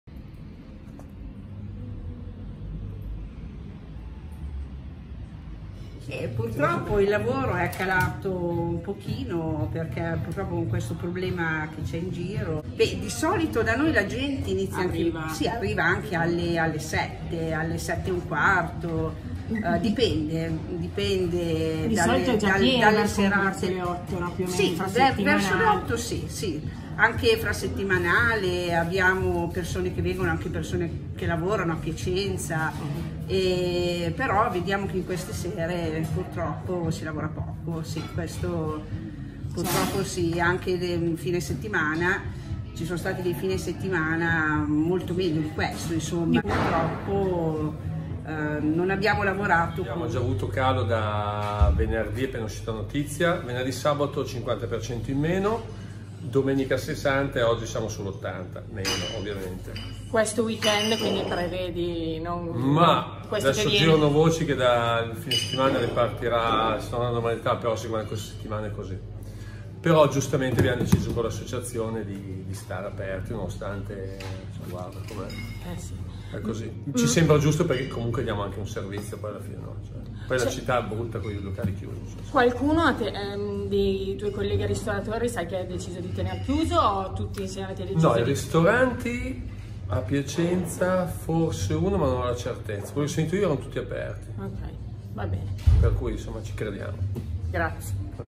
CORONAVIRUS, IL MARTEDÌ MAGRO DEI RISTORATORI PIACENTINI
coronavirus Piacenza Il martedì magro dei ristoratori piacentini: “ Come categoria abbiamo voluto restare aperti per dare un segnale positivo. Siamo cittadini ancora prima che imprenditori”. Locali quasi vuoti alle 20.30 del martedì di